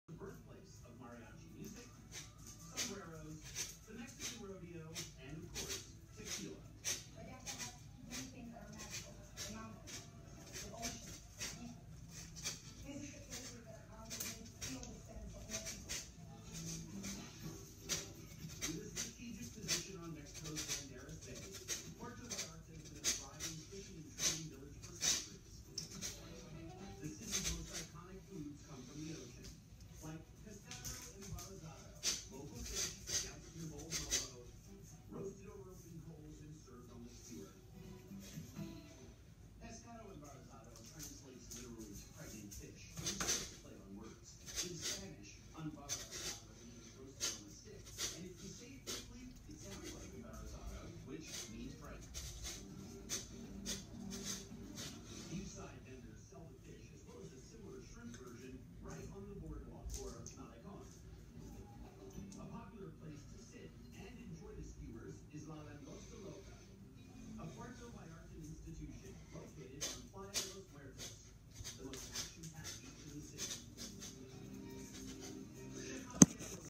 jeet kune do Kung fu, JKD
Jin Choi, ping Choi, I prefer uppercuts to hooks, never hook with a hooker, bullshit, any pro coach will tell you that if you can’t escape then you have to hook on the inside, I love jeet kune do, it’s an effective and amazing art
Much respect to the founder, grandmaster Bruce Lee, Jun Fan Chinese boxing